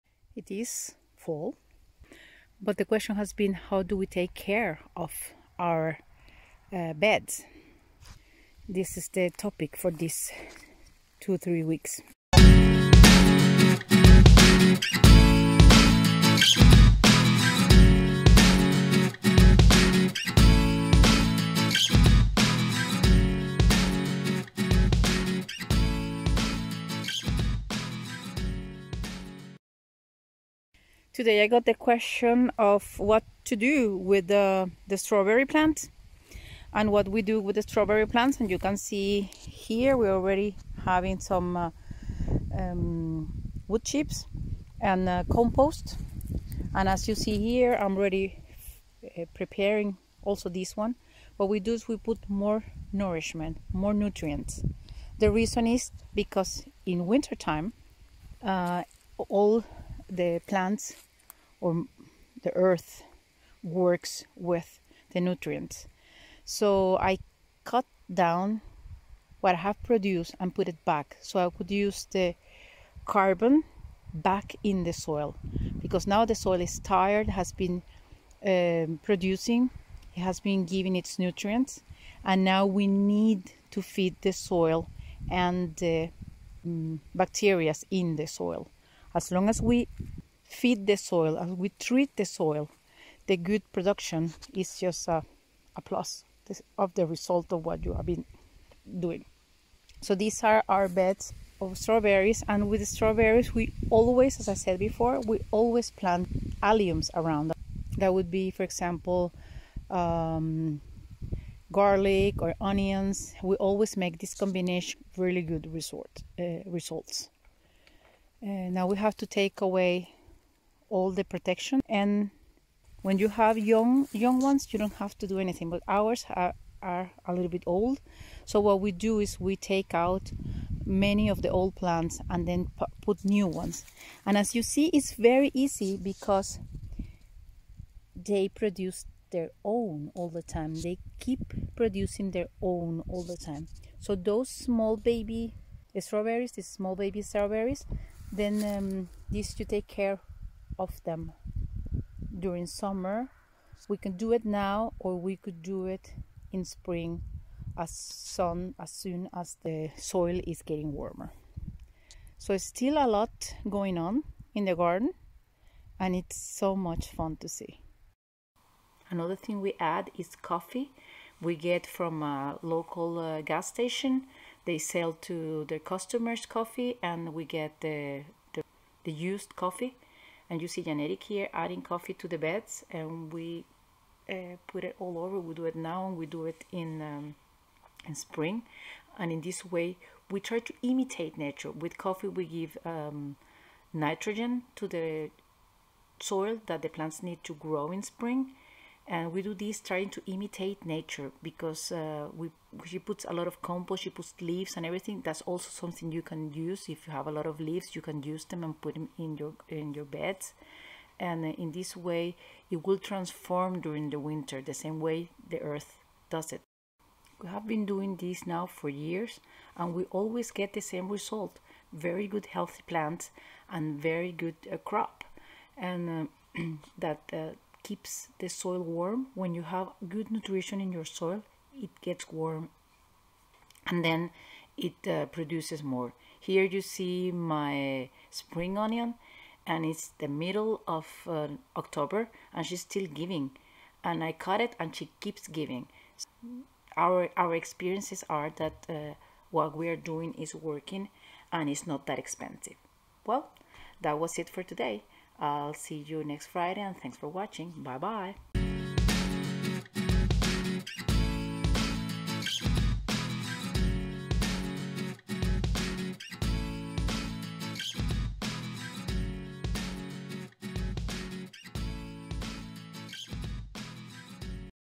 Preparing our beds to winter and spring part 1
Use the power of the seasons. A short introduction on how to pepare your beds for winter and spring